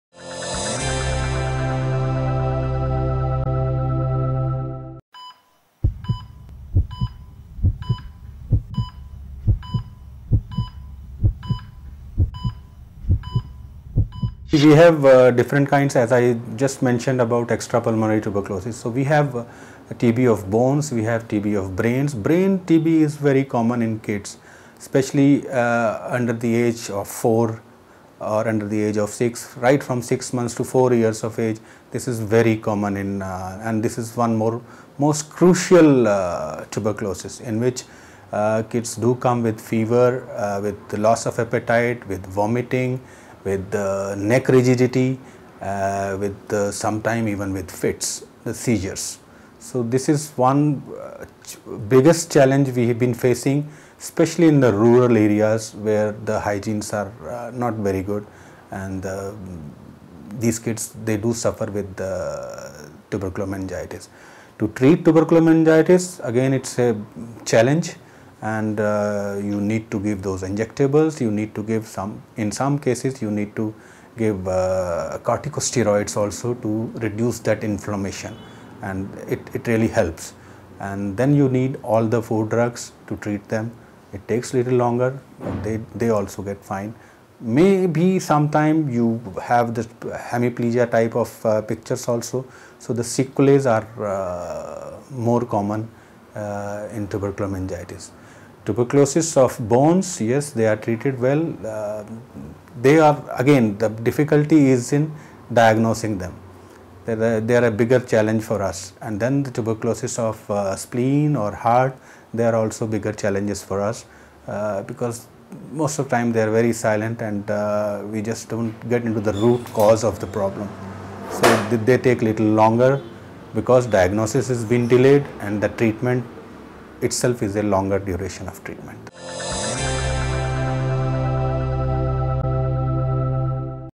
Different Types of Tuberculosis - Onlymyhealth.com
For Similar Videos Click

Tuberculosis is an infectious disease which generally attacks lungs but sometimes can affect other parts of the body. There are different types of TB which includes bone and brain TB. Among the two, bone TB is very common and crucial in kids between 0-4 years of age which results in fever, loss of appetite, neck rigidity and vomiting. A large number of kids in rural areas suffer from tuberculosis meningitis due to the poor hygiene conditions. It can be treated effectively with food drugs available in the market. The diagnosis of bone, spleen and heart tuberculosis is difficult as it takes a long period of medication to treat them.